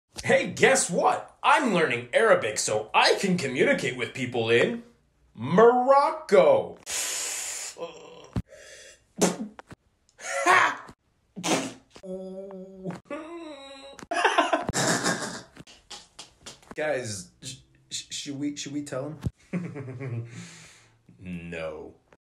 Arab world be like….
That Darija dialect man….

(to be fair, other N. African countries are not too far off, but you guys have told me Moroccans have the thickest accent)